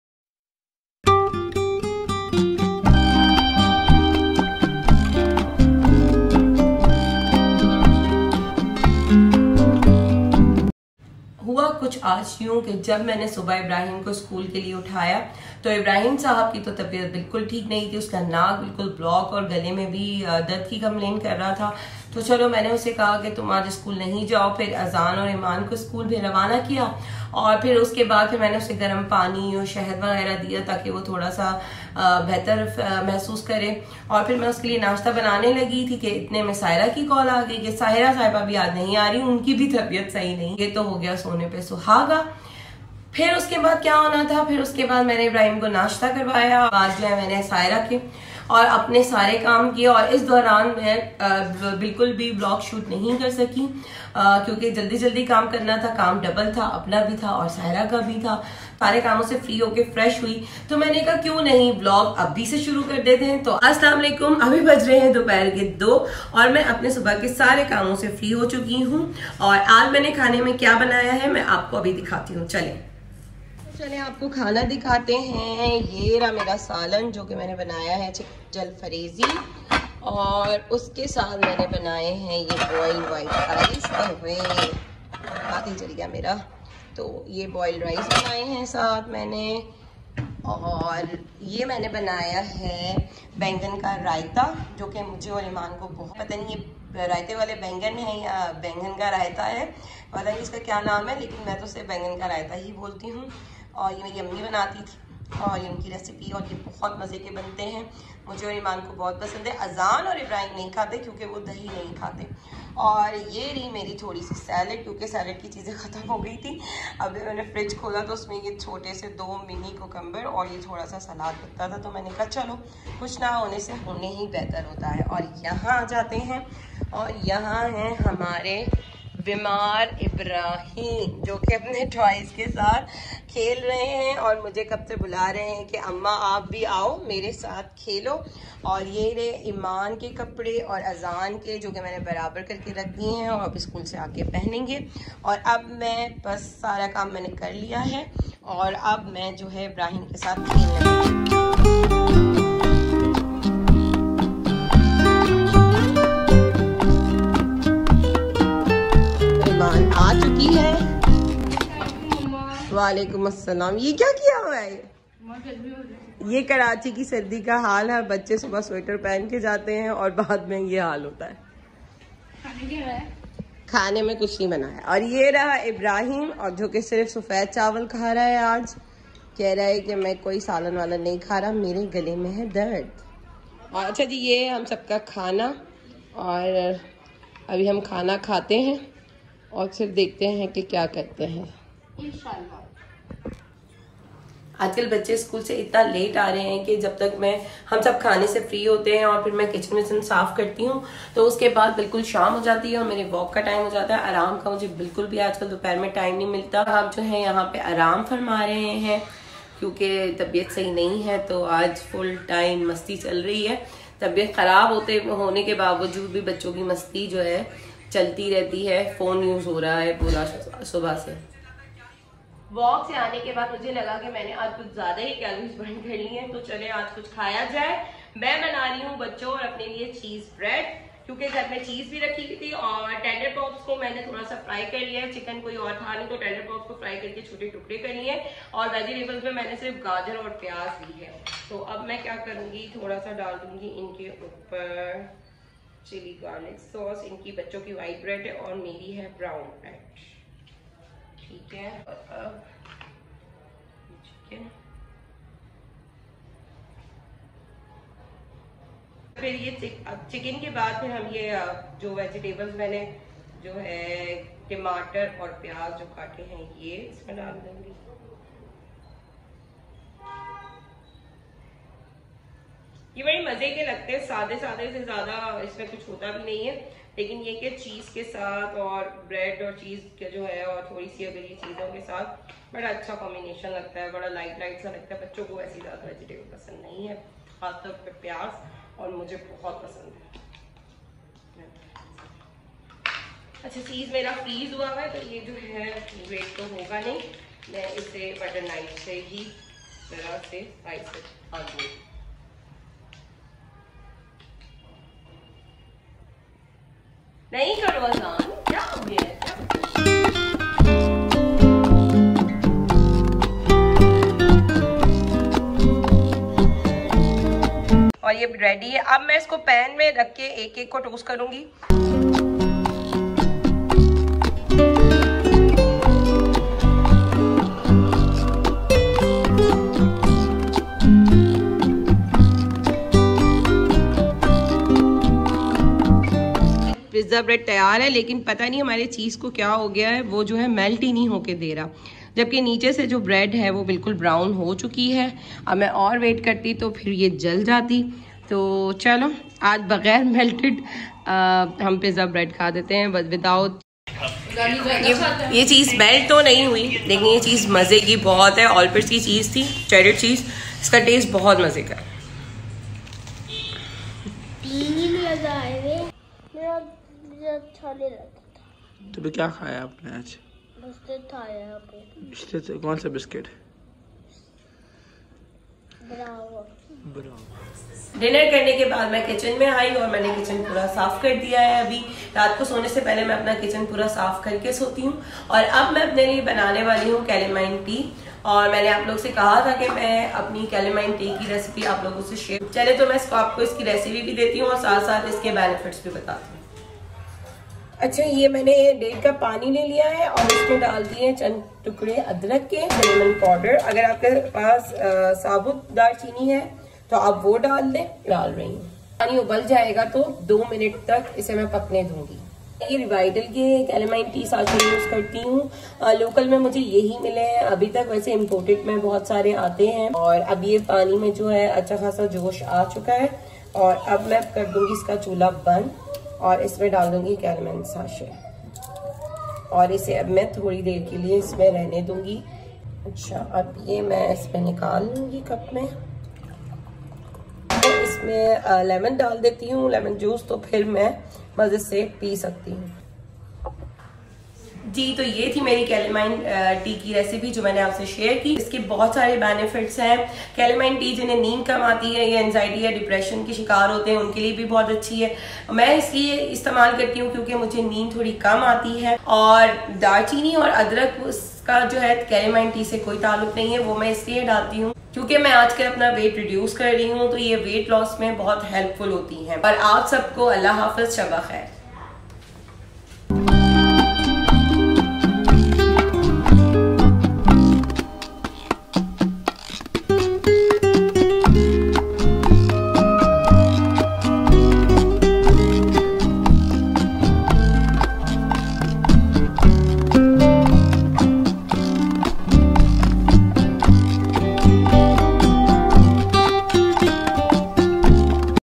Master the art of cooking Bread Pizza & Chamomile Tea
Join us in this video as we teach you how to make the perfect bread pizza and chamomile tea.

Whether you're a seasoned cook or just starting out, this recipe is easy to follow and packed with flavor.

In this step-by-step tutorial, we'll show you how to make a delicious bread pizza with your favorite toppings and the perfect chamomile tea to pair with it.

So grab your apron and let's get cooking! You'll love the end result and will want to make it over and over again.

Love you all, Sadaf Mirza